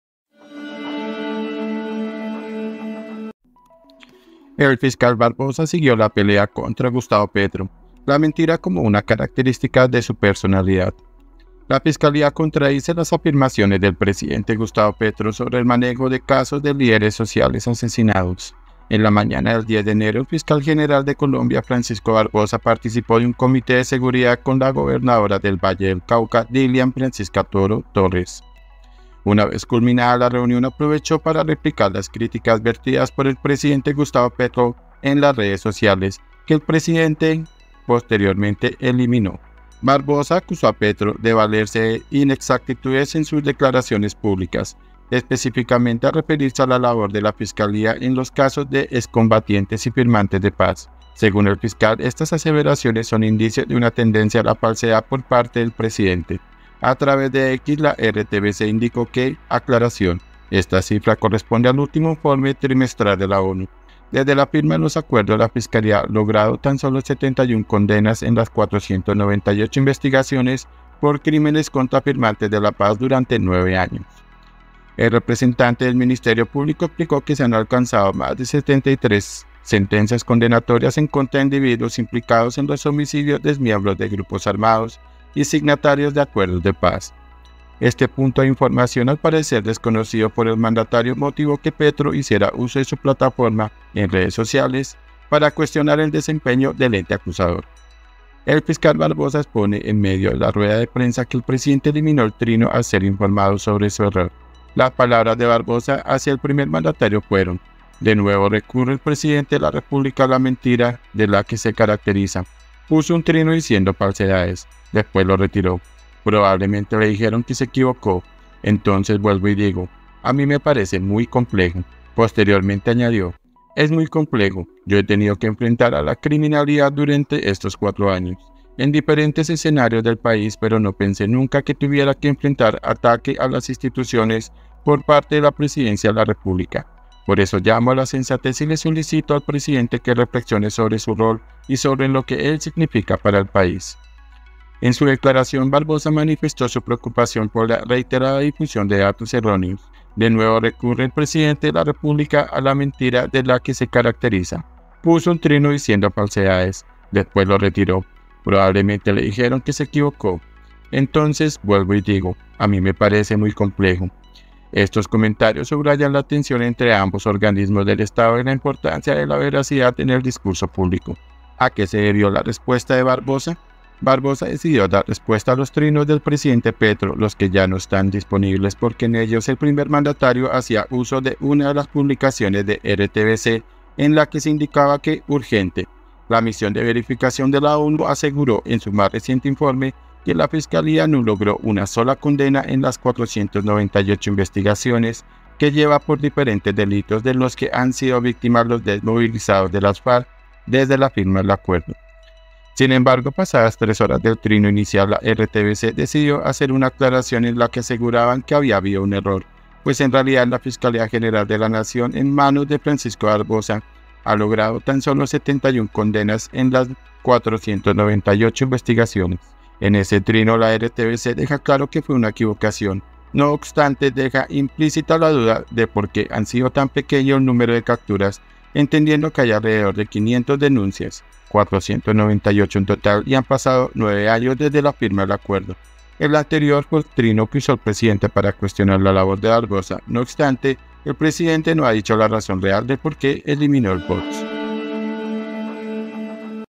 ¿POR QUÉ LO BORRÓ? 🔴 El fiscal Barbosa siguió la pelea contra Gustavo Petro
En la mañana del 10 de enero, el fiscal general de Colombia, Francisco Barbosa participó de un comité de seguridad con la gobernadora de Valle del Cauca, Dilian Francisca Toro Torres.
INFOBAE